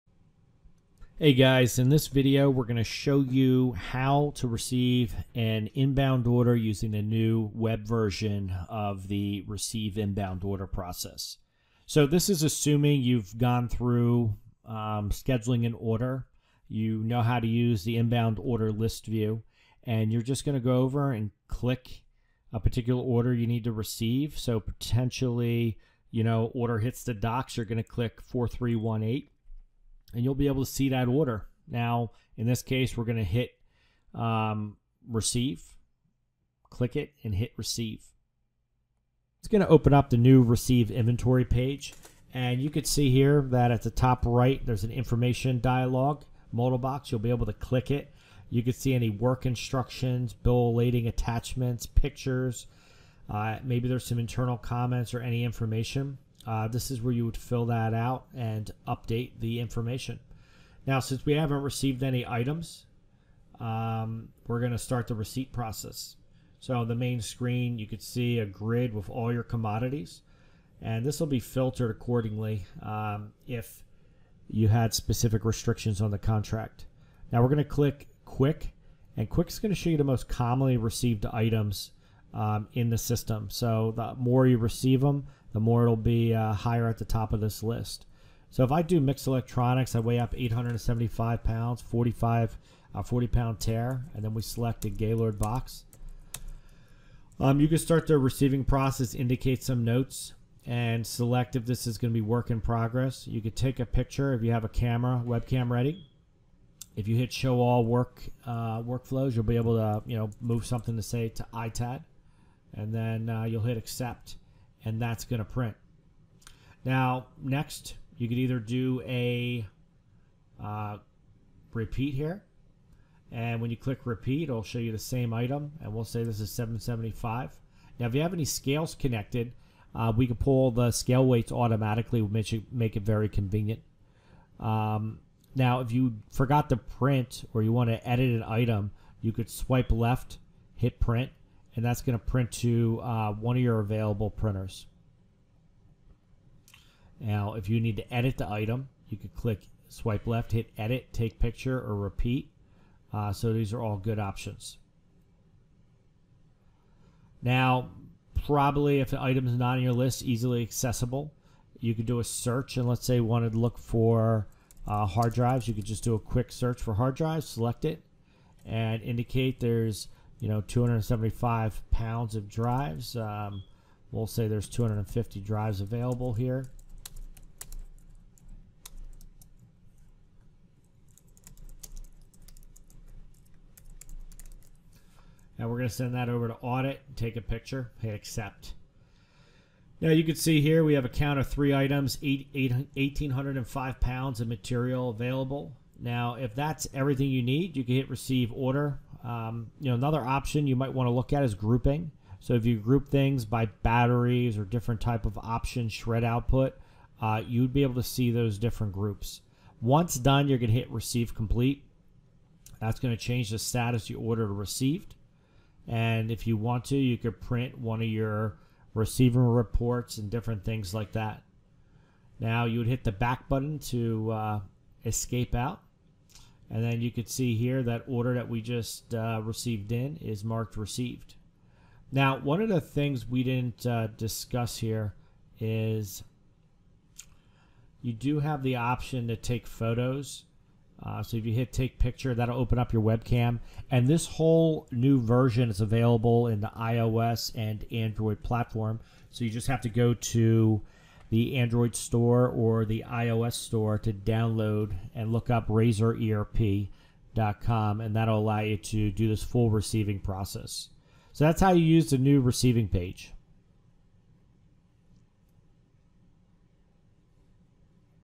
How To Use The New Receive Inbound Order Screen
🚀 Mastering Inbound Orders with Razor ERP's New Receive Screen! 📦

Discover the game-changing features of Razor ERP's latest innovation – the Receive Inbound Order Screen! 🌟 In this tutorial, we'll guide you through an effortless journey of understanding how to utilize this cutting-edge tool to streamline your order management process.

🎯 Ease of Use: Say goodbye to complex interfaces! We'll show you how the new Receive Screen is designed with user-friendliness in mind, making order processing a breeze for both beginners and seasoned professionals.

📱 Responsive Touchscreen Integration: Experience the future of efficiency as we delve into the seamless touchscreen integration. Learn how to navigate and interact with the screen using intuitive touch gestures, enabling you to handle orders on your preferred device.

⚙️ Scalability at its Best: Whether you're dealing with a handful of orders or a massive influx, Razor ERP's Receive Screen scales effortlessly. Join us to explore how this feature adapts to your business's growth, ensuring you're always equipped to meet demand.

Don't miss out on this opportunity to elevate your order management game with the power of Razor ERP's innovative tools. Watch the tutorial now and revolutionize the way you handle inbound orders!